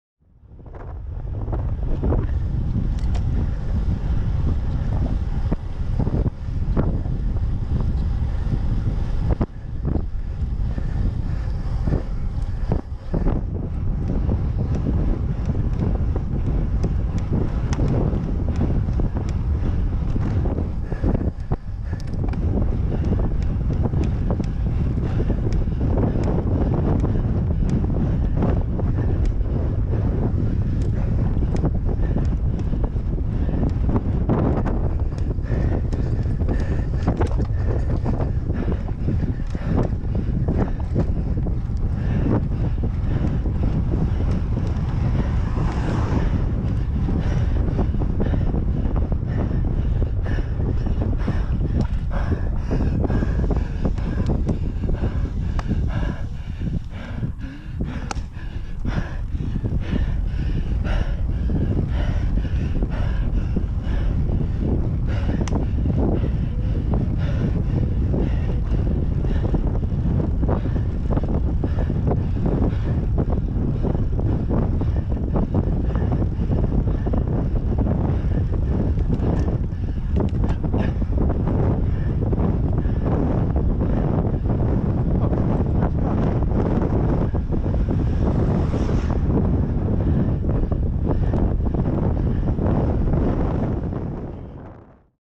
Grendon Grind
Strava segment called Grendon Grind in Northamptonshire. Average gradient 4%,steepest bit 20%.